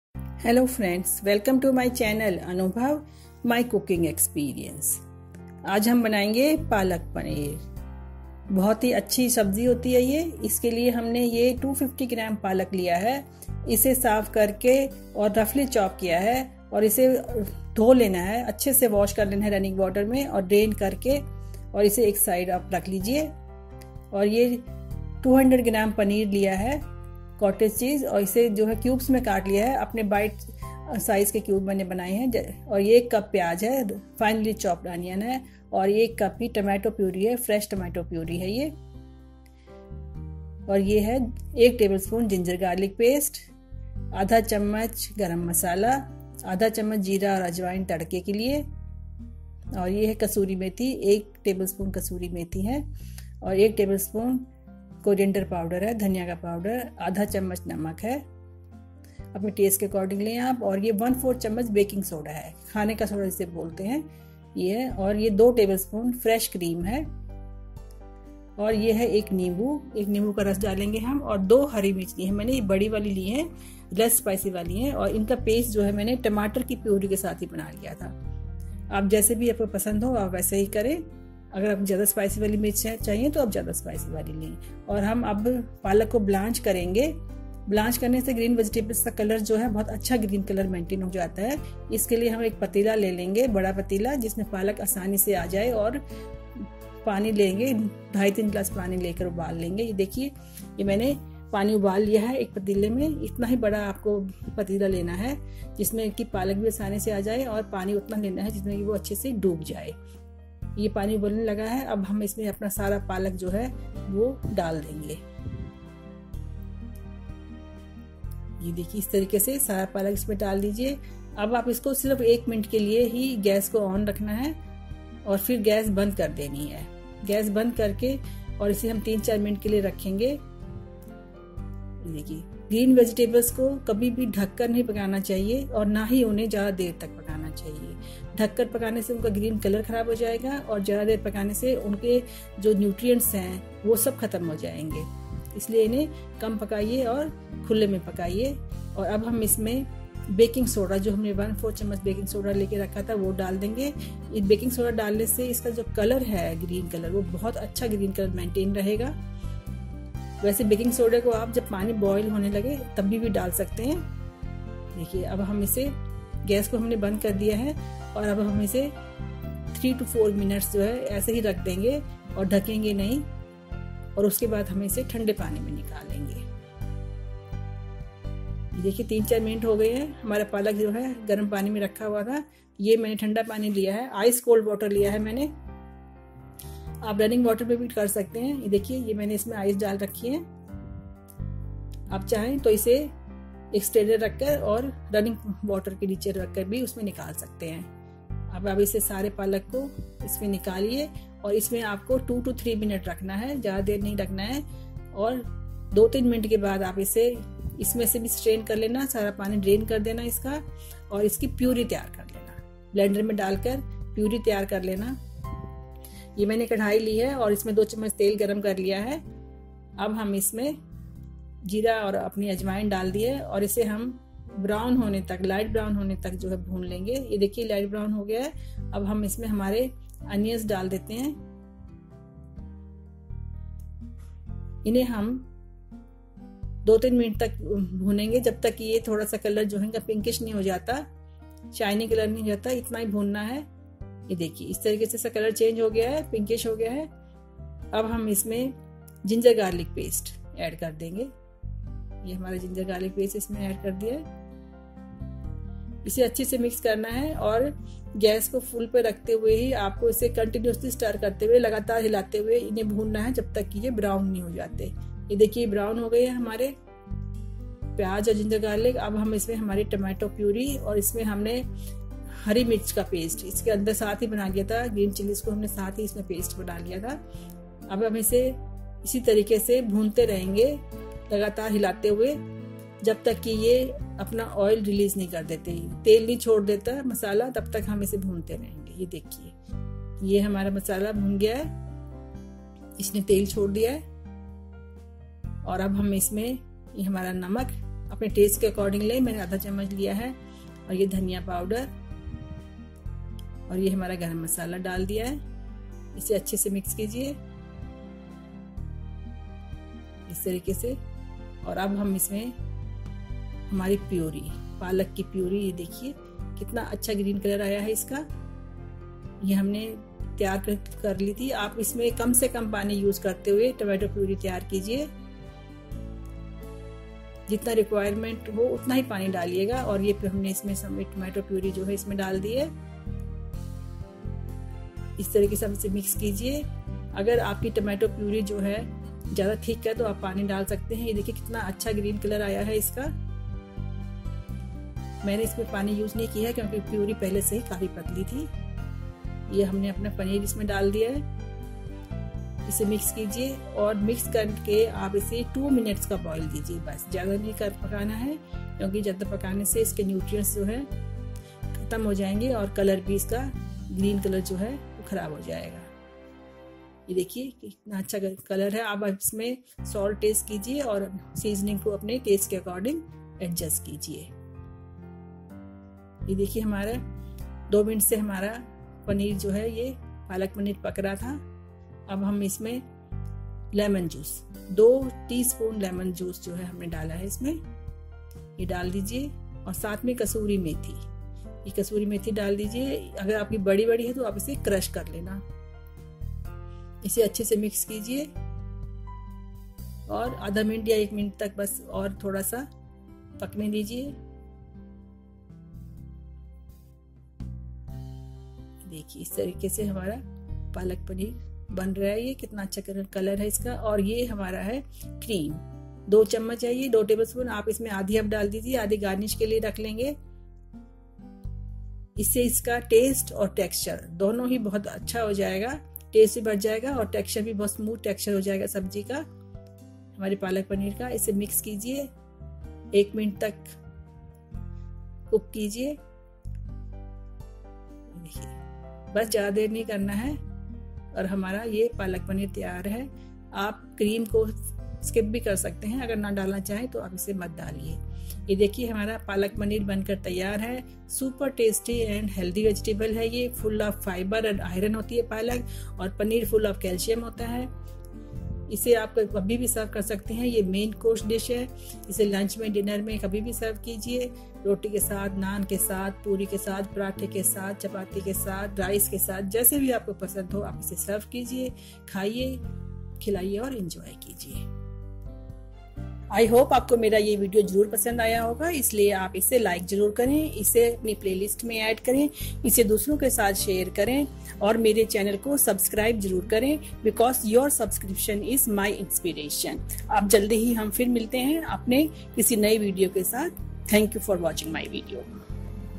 Palak Paneer | पालक पनीर | Delicious and healthy | With English Subtitles
palak paneer is a famous Indian dish. It is made with Indian cottage cheese and spinach. It is delicious, healthy and rich in iron, calcium and protein. Enjoy this dish with rice and roti.